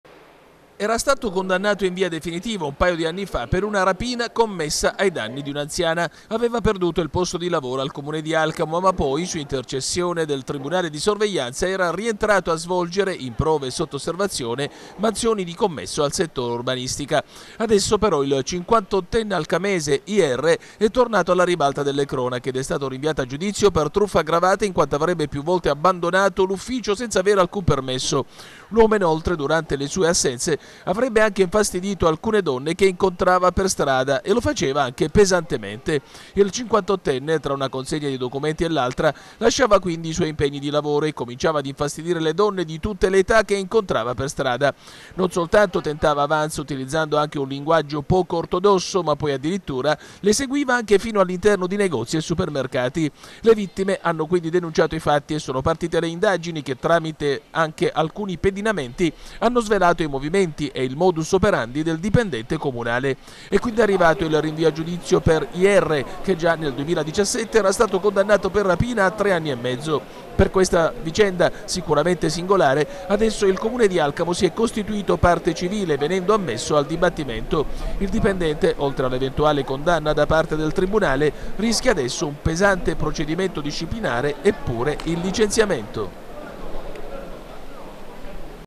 Alcamo. Infastidiva donne in strada, 58enne rinviato a giudizio
Lasciava gli impegni di lavoro e infastidiva pesantemente le donne in strada, anche pedinandole. Un dipendente 58enne del comune di Alcamo è stato quindi rinviato a  giudizio. Aveva già perso il posto di lavoro, poi reintegrato, dopo una condanna per rapina.